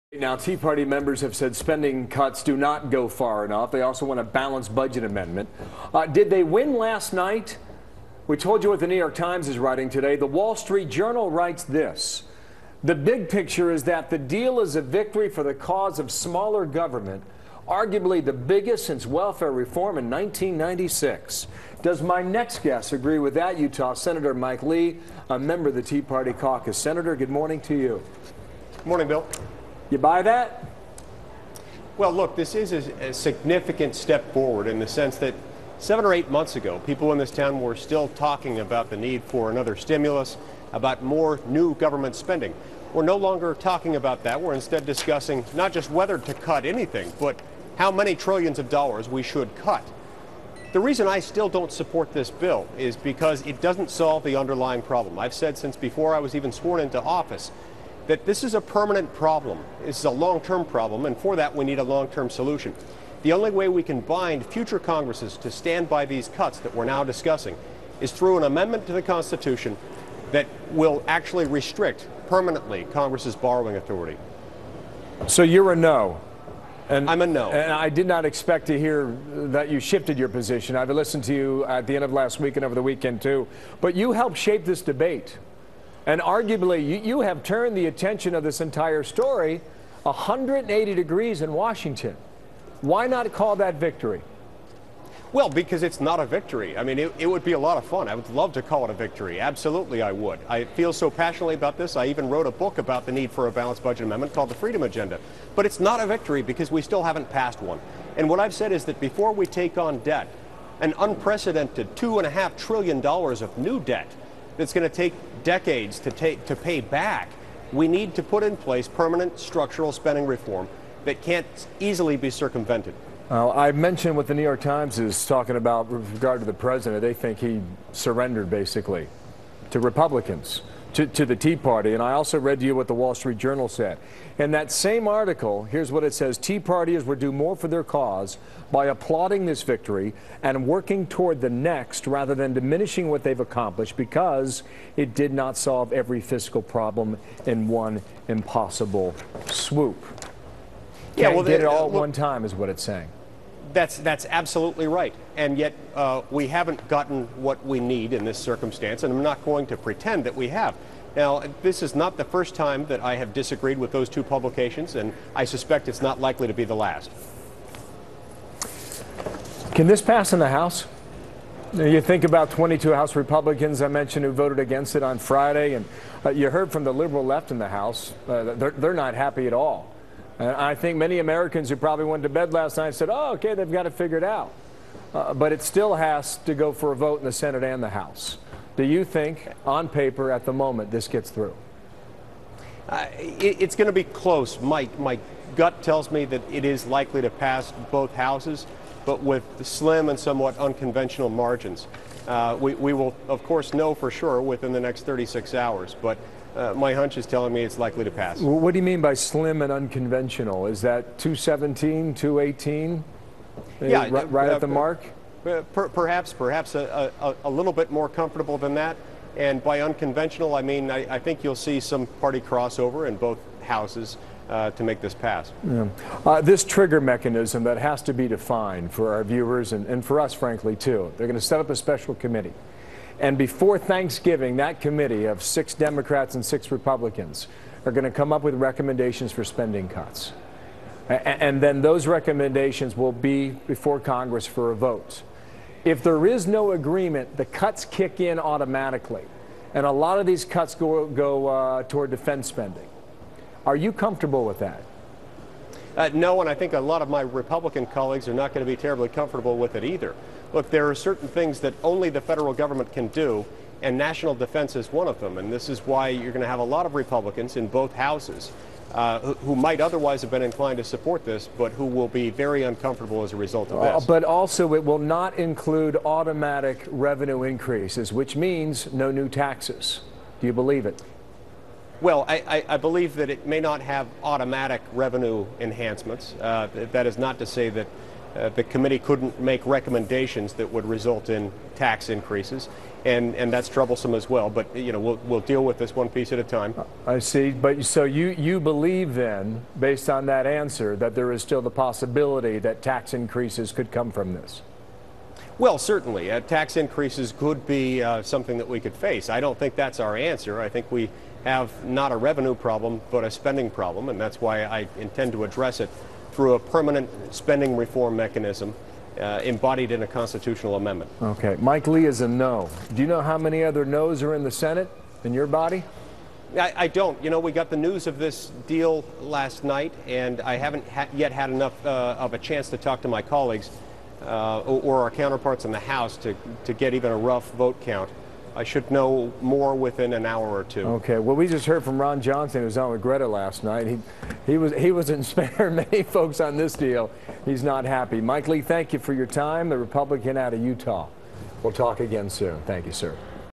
Senator Mike Lee is a No on the Debt Ceiling Deal
Without structural spending reform, Senator Lee positions himself to vote no on the latest debt ceiling deal.